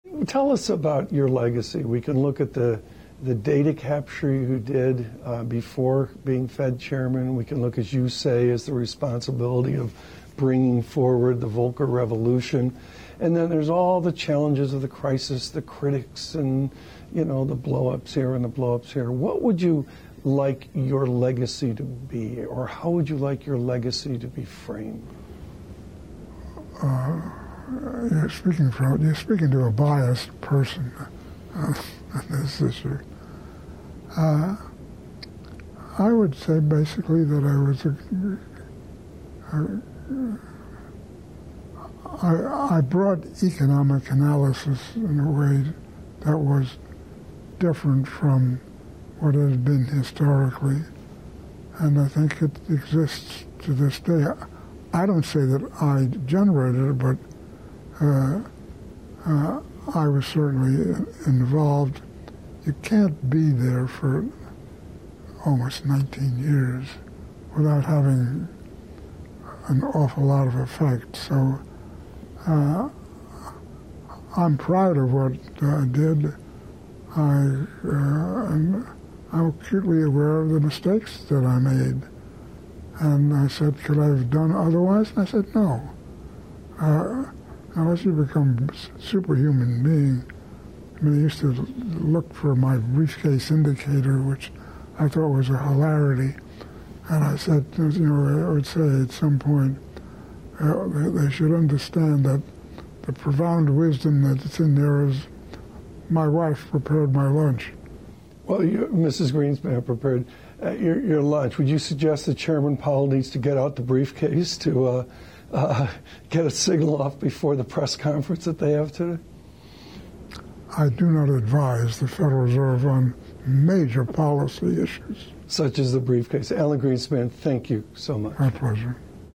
Alan Greenspan Reflects on His Legacy
Jan.07 -- Former Federal Reserve Chairman Alan Greenspan reflects on his legacy during an interview with Bloomberg's Tom Keene.